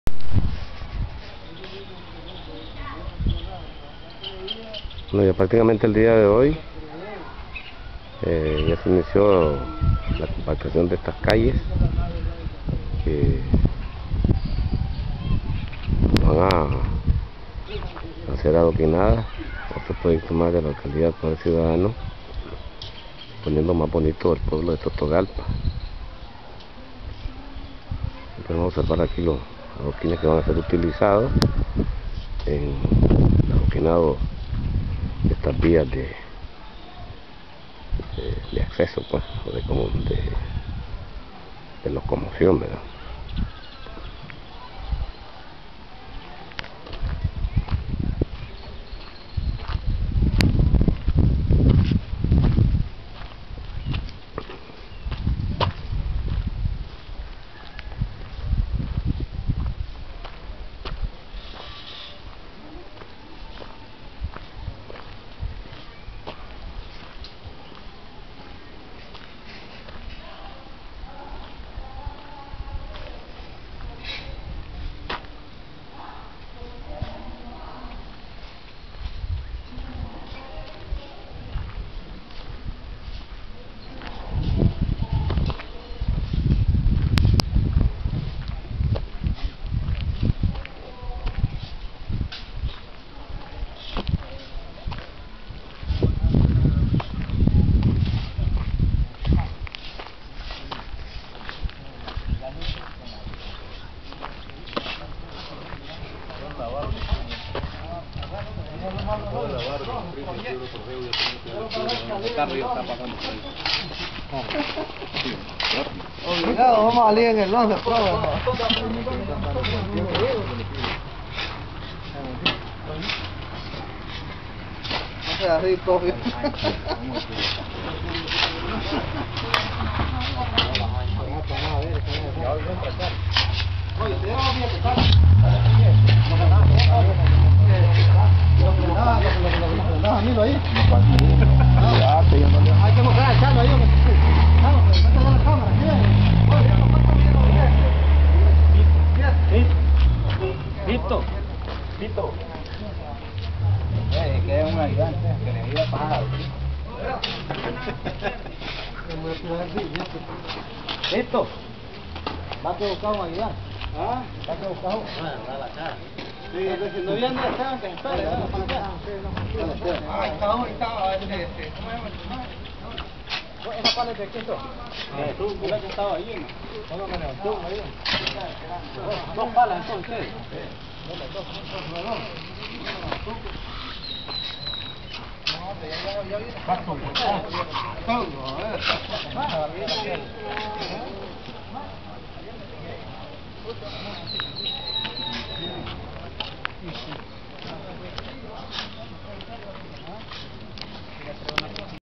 Adoquinamiento de dos calles.
Continua cambiando Totogalpa, dos calles seran adoquinadas, ya se inicio el dia de hoy 26 de abril, estas pertenecen al Barrio Germán Pomares Ordoñez; el Gobierno del Dr. Melvin López Gadea, haciando realidad las promesas de campaña que lo llevaron a la silla edilicia.
Seguimos cambiando nuestro Municipio; con Daniel Presidente 2011..Seguimos Cambiando Nicaragua.